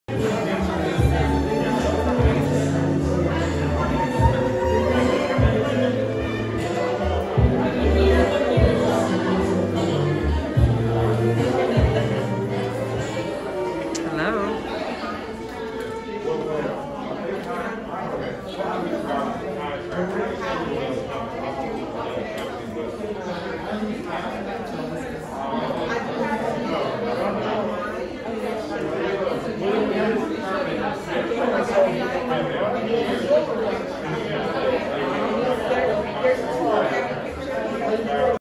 LIVE! From The FSC: Hayti's 3rd Friday Art & Business Walk Dec 2021
BTS at 6:30 PM Warming it up in the Lyda Moore Merrick Gallery and the Claronell K. Trapp Brown Community Room. Artist Market and We Are Here: Past, Present, Future curated by Gail Belvett. David Scott Binanay on violin. Tyra Dixon, videographer.